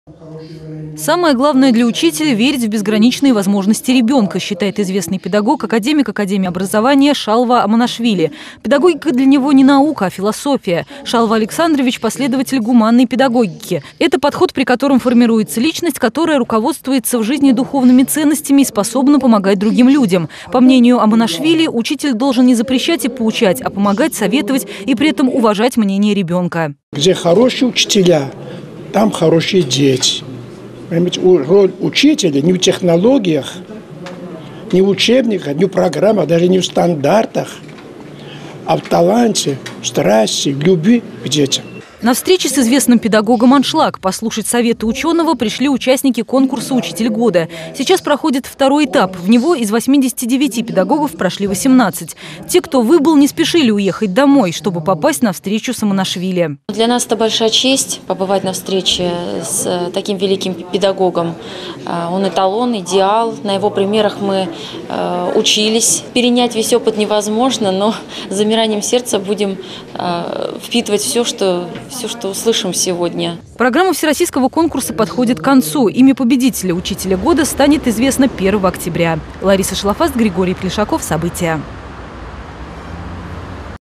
Участники конкурса "Учитель года" встретились с академиком РАО Шалвой Амонашвили
На встрече с известным ученым аншлаг. Послушать советы ученого пришли участники конкурса "Учитель года".